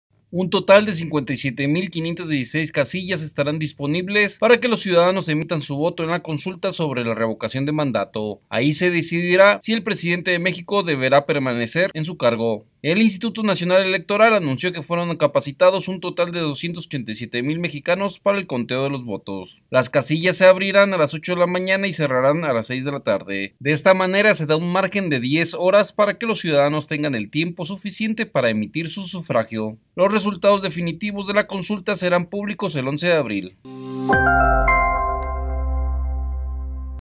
57,516 casillas para consulta
Un total de 57,516 casillas estarán disponibles para que los ciudadanos emitan su voto en la consulta sobre la revocación de mandato. Ahí se decidirá si el presidente de México deberá de permanecer en su cargo.

El Instituto Nacional Electoral (INE) anunció que fueron capacitados un total de 287 mil mexicanos para el conteo de los votos.

Las casillas se abrirán a las 8 de la mañana y cerrarán a las 6 de la tarde. De esta manera se da un margen de 10 horas para que los ciudadanos tengan el tiempo suficiente para emitir su sufragio.

Los resultados definitivos de la consulta se harán públicos el 11 de abril.